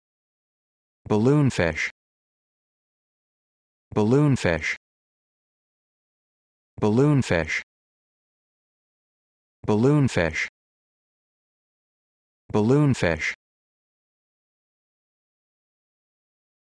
How to Pronounce Balloonfish
This video shows you how to pronounce Balloonfish